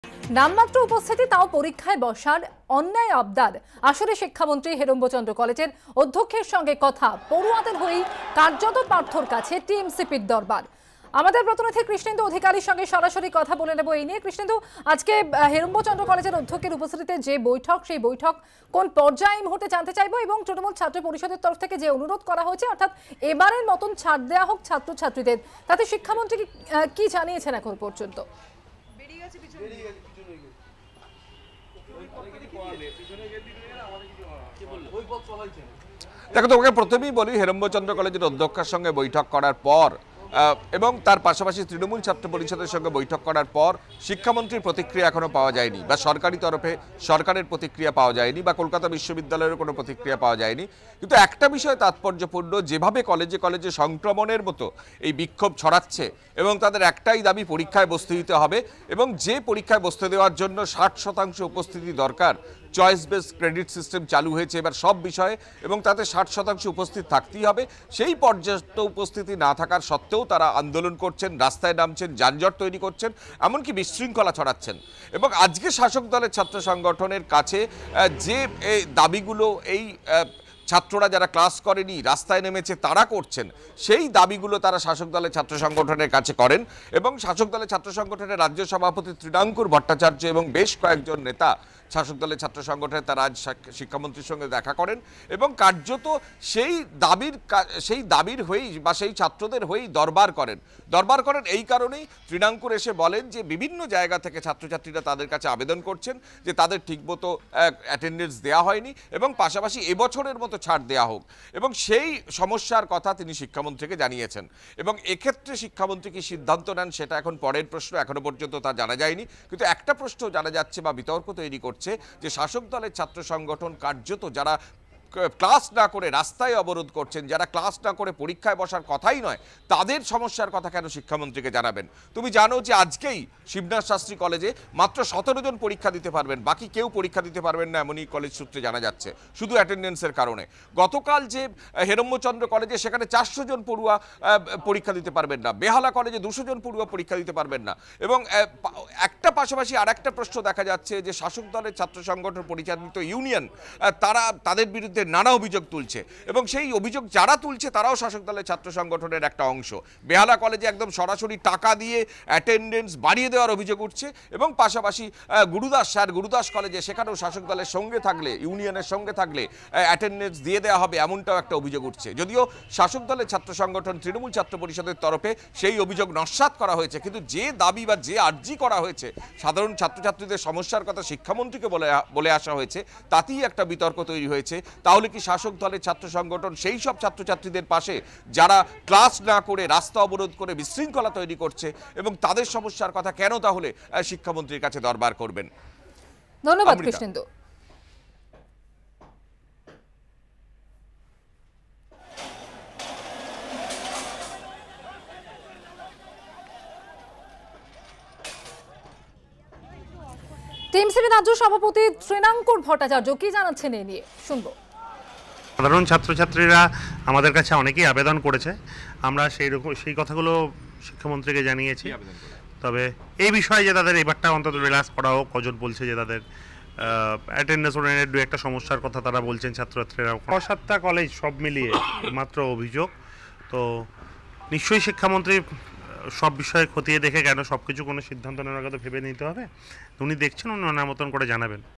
নামমাত্র উপস্থিতি, তাও পরীক্ষায় বসার অন্যায় আবদার, আসরে শিক্ষামন্ত্রী | ABP Ananda
Education Minister Partha Chatterjee talks to the Principal of Heramba Chandra College on demand of students seeking the permit to sit for exam despite low attendance.